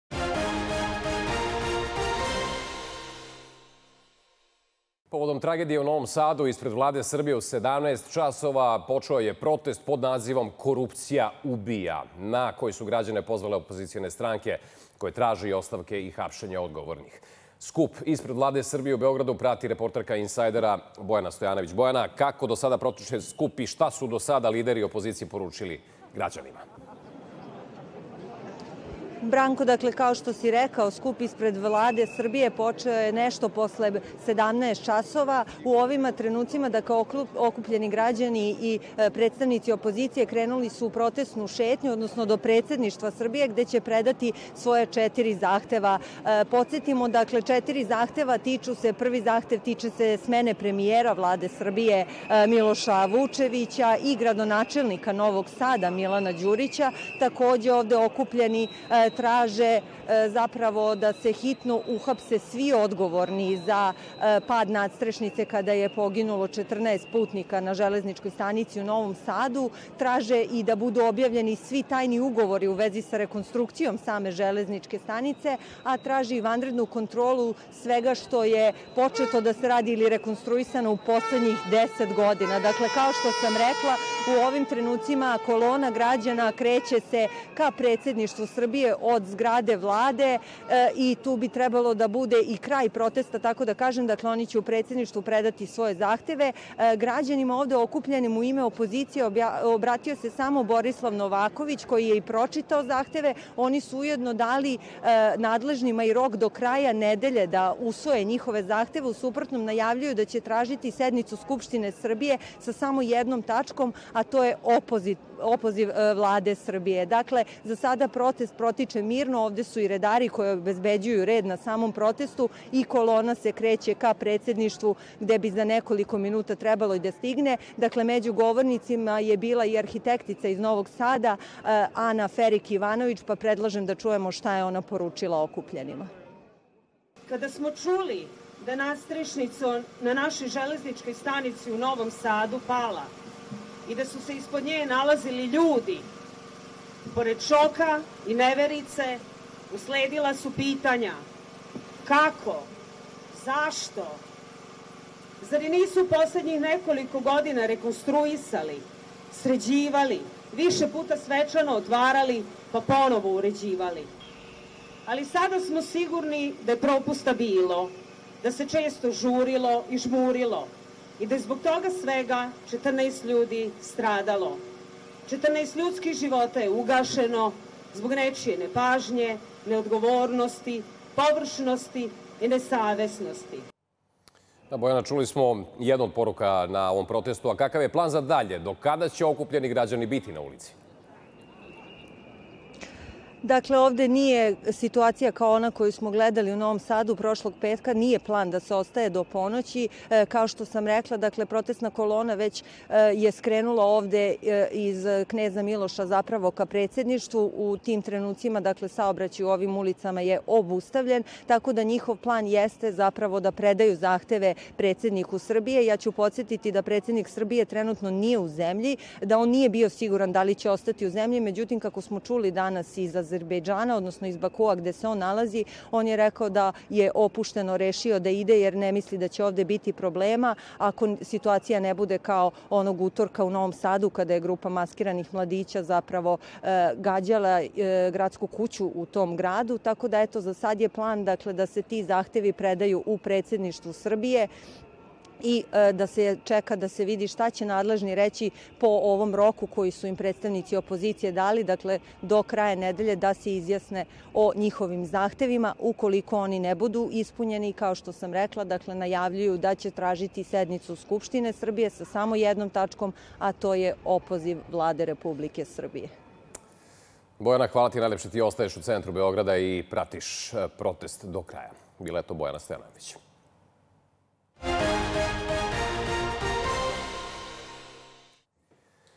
Reporterka Insajdera sa protesta ispred Vlade Srbije
U Beogradu je održan protest opozicionih stranaka "Korupcija ubija" ispred zgrade Vlade Srbije, povodom tragične nesreće na Železničkoj stanici u Novom Sadu, gde je urušavanje nadstrešnice dovelo do pogibije 14 osoba. Okupljeni građani i predstavnici opozicije zatražili su ostavke odgovornih zvaničnika, ističući neophodnost preuzimanja odgovornosti za ovu tragediju, a umesto Predsedništvu, zahtevi će biti predati Narodnoj skupštini, sa rokom od nedelju dana da budu ispunjeni. Protest je protekao mirno, uz paljenje sveća u znak sećanja na stradale.
Pogledajte kompletan izveštaj reporterke Insajdera Bojane Stojanović.

Pratite nas na i na društvenim mrežama